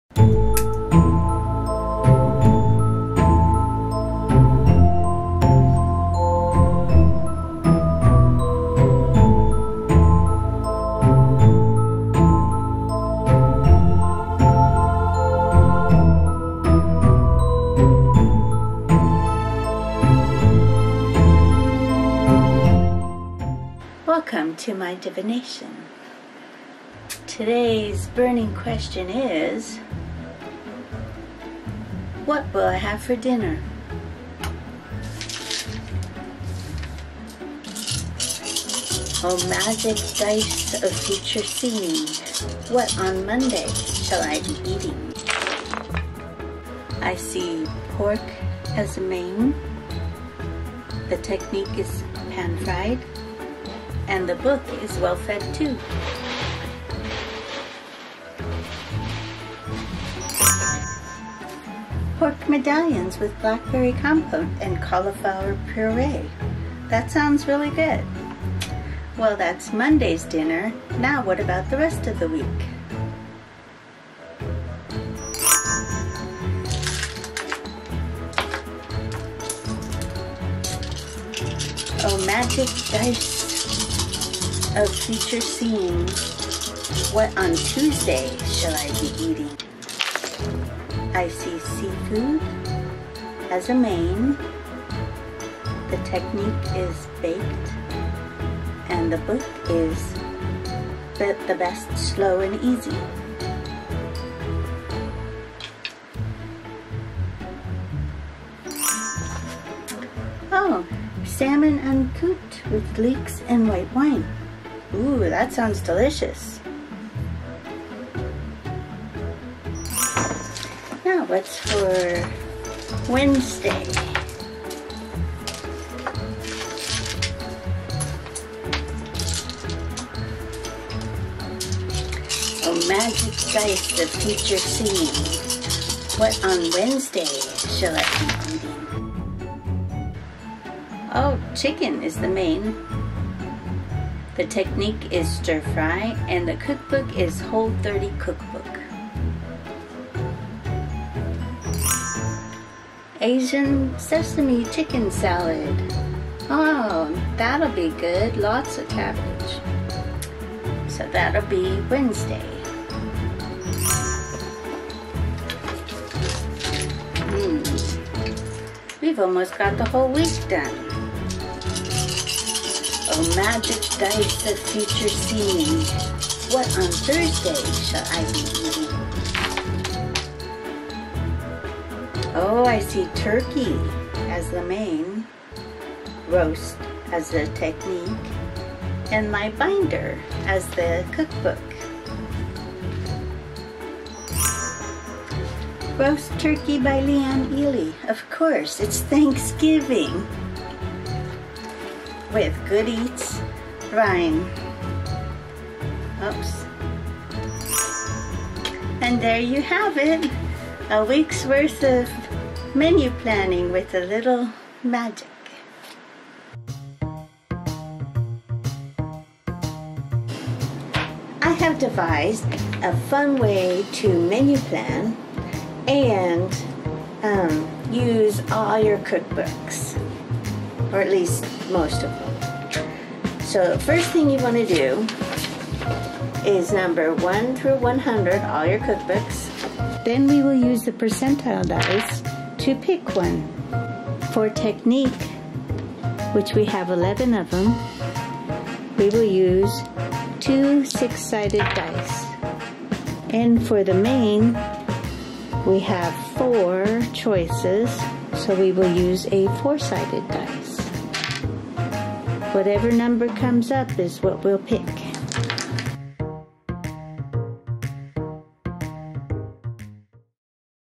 Deena Witch Sees The Future
I came up with a fun way to menu plan which I explain towards the end of the video and I added a little witchy flair to it!

You need percentile dice, two 6 sided dice and one 4 sided die.  Then label your cookbooks 1 - 100,  The techniques are:  1: nothing 2: pan fry  3: steam  4: boil  5: stir fry  6: roast  7: braise 8:casserole  9: Bake  10: instapot
11: sous vide  12: crockpot.  The 4 mains are:  1: Beef  2: poultry  3:  seafood and 4: Pork.    You can make the categories anything you would like, if you want to change it up.